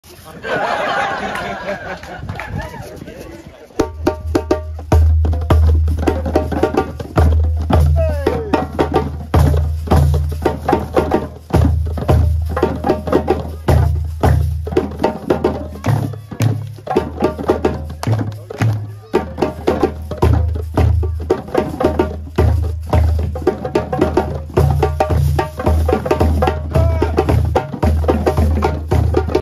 Drumming Circle on Juneteenth at Casey Farm, Sunday June 19, 2022
The crowd participated in the djembe drumming performance by Sidy Maiga and family at the June 19 dedication event.

Read more about Casey Farm's history here:  ow.ly/CszY50KjblL